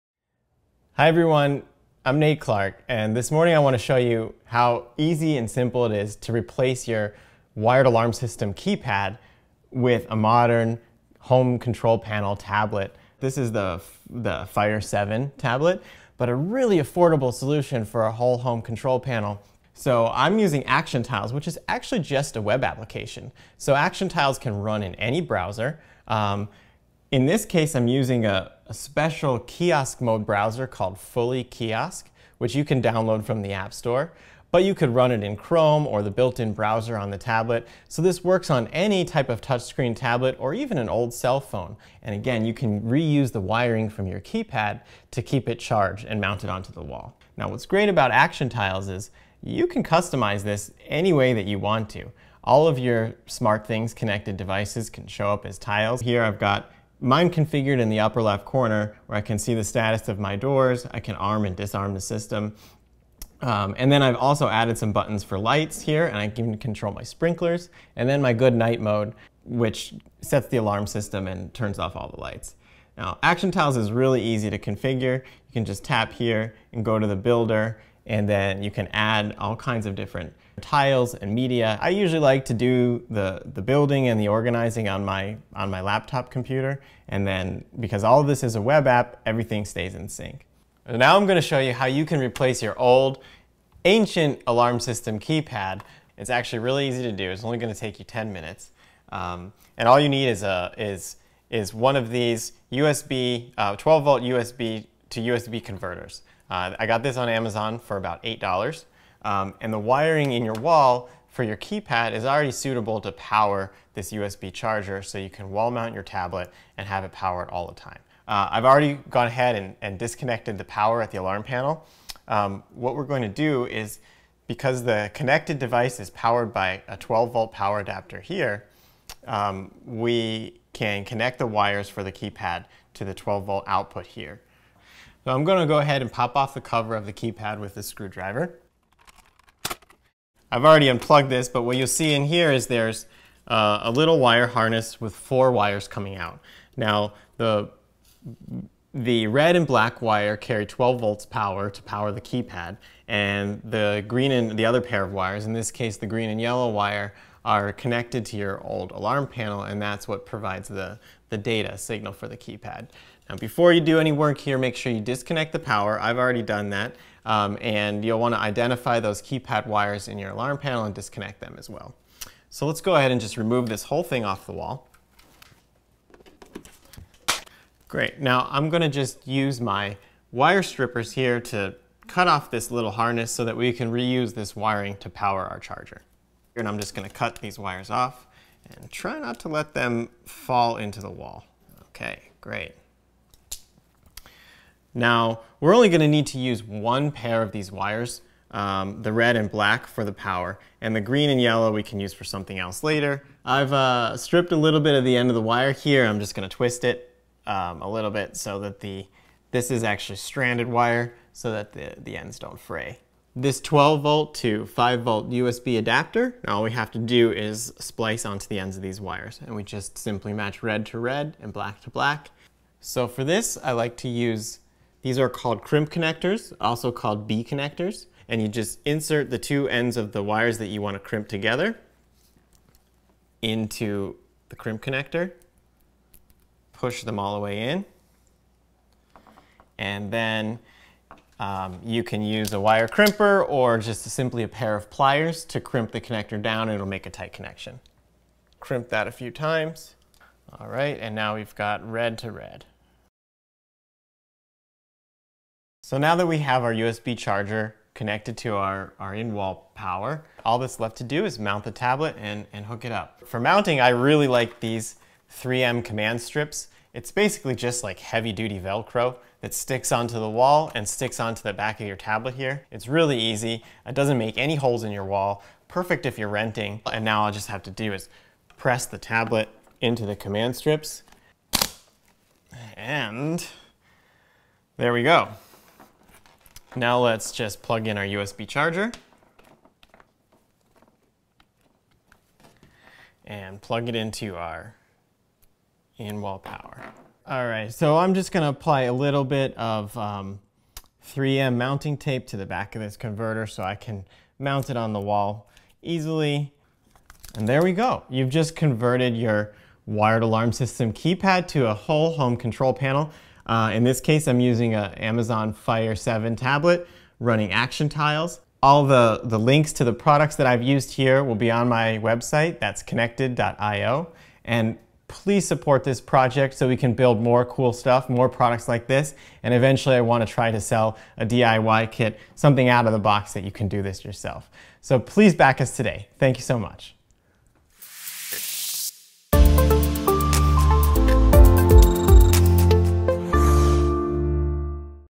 Replace your alarm system keypad with an ActionTiles touch-screen tablet — Konnected DIY add-on
You've modernized your old wired alarm system with the Konnected Alarm Panel and now you want to update those ugly wired alarm system keypads. It's easy and can be done for less than $100!

Products shown in this video:
Amazon Fire 7 tablet
3M Command Strips
Wire stripper and crimp tool
3M Scotch mounting tape